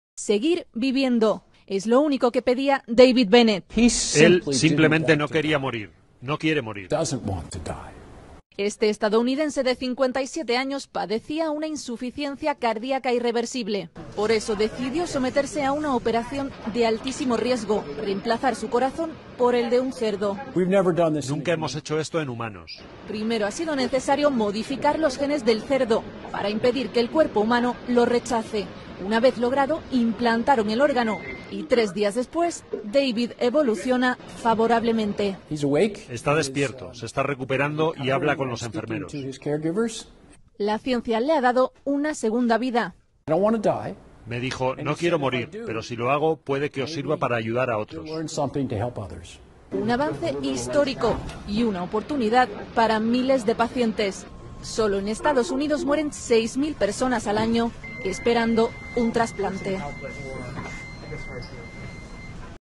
Transplantan el corazón de un cerdo modificado a una persona | Notas Internacionales.
REDES CANAL 7TVMX

REDES DEL CONDUCTOR